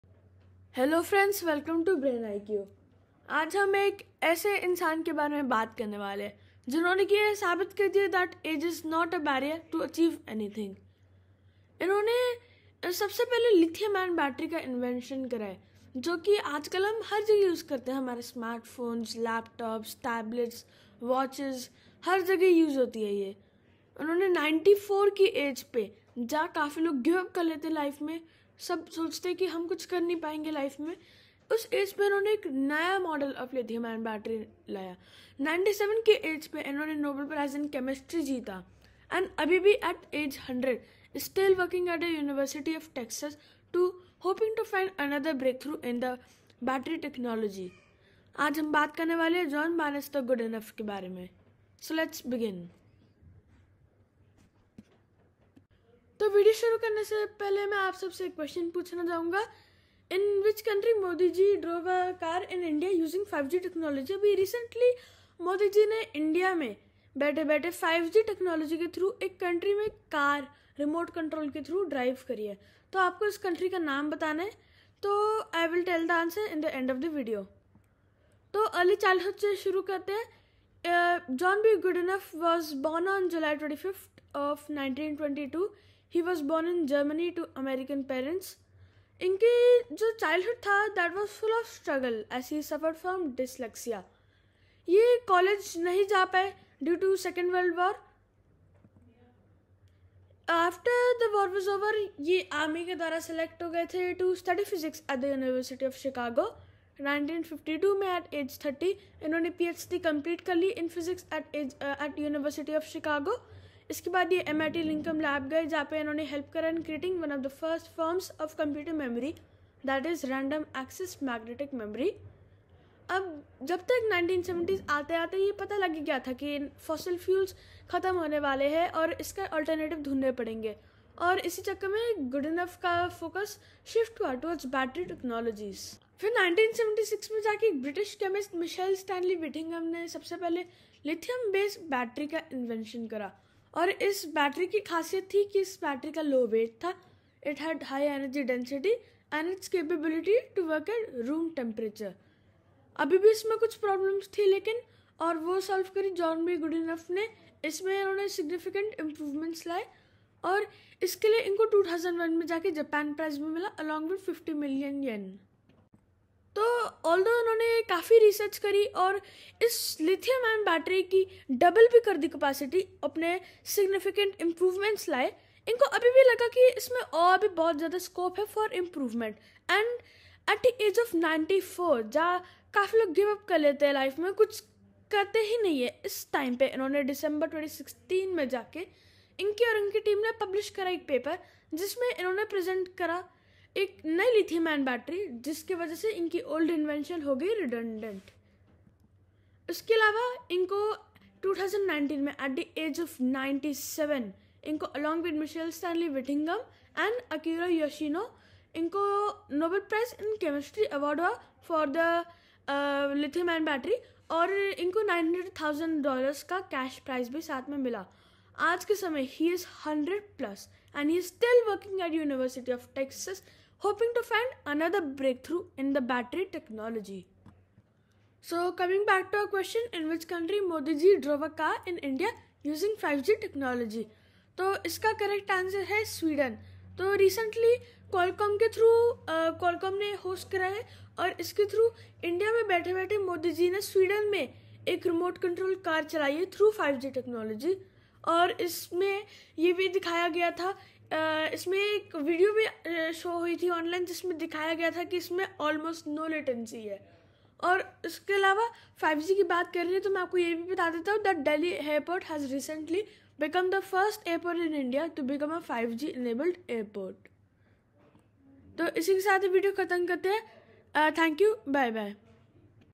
John Bannister Goodenough - Won Nobel Prize at age 97 || Saubhagya Lohia || Brain IQ
This video covers the topic of John Bannister Goodenough and covers the sub-topics as follows:
1) Early Childhood
2) Timeline
3) Shift to Battery technology
4) Nobel Prize

00:00 Intro
00:55 Question
01:18 EarlyChildhood
01:39 Timeline
02:16 ShiftToBatteryTechnologies
03:19 NobelPrize
03:48 Question
04:28 Outro